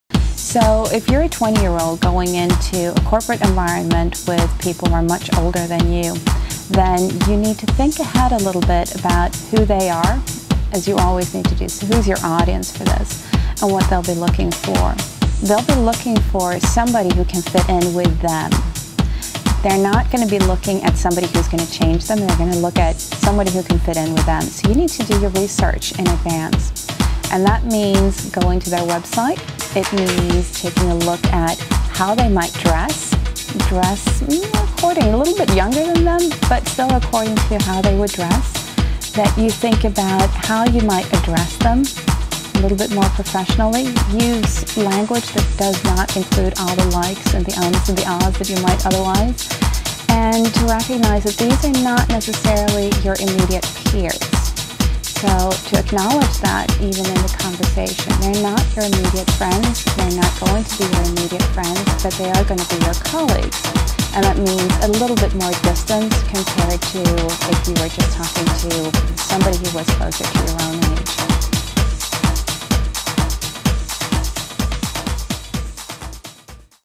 Generation Y vs Baby Boomers Communication in the Workplace
How does communication in the workplace differ between  generation Y and the baby boomers? Get career advice on how to bridge the generation gap and polish your professional communication.